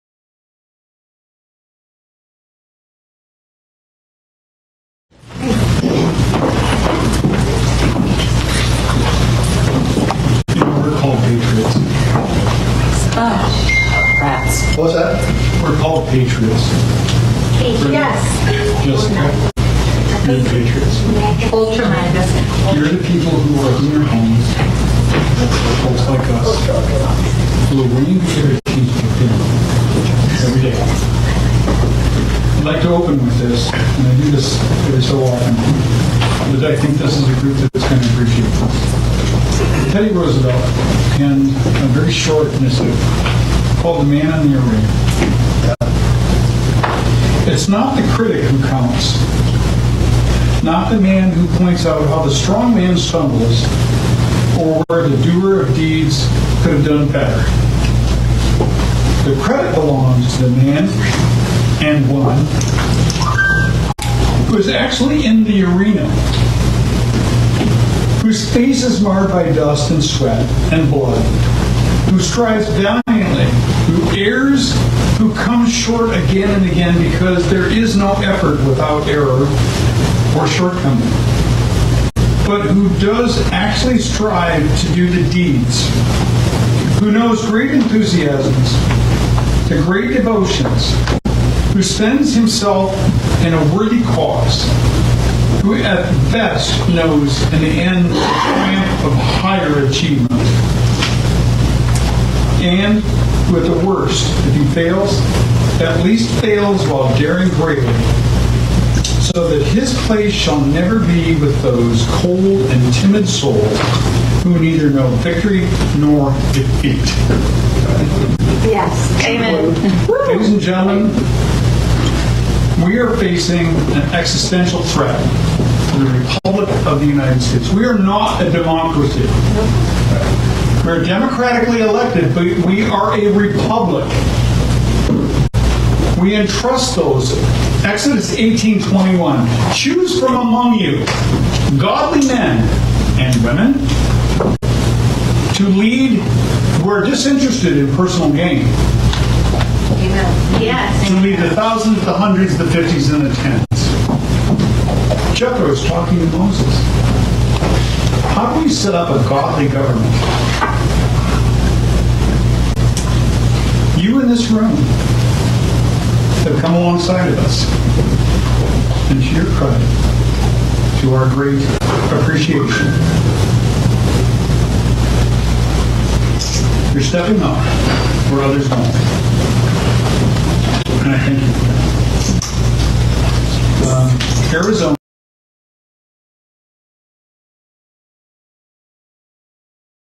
MARK FINCHEM Recites The Man in the Arena – Teddy Roosevelt historic speech 9/2022 VFAF
MARK FINCHEM Veterans For America First endorsed candidate Recites The Man in the Arena – Teddy Roosevelt historic speech 9/2022 , Filmed by Stan & Donna Fitzgerald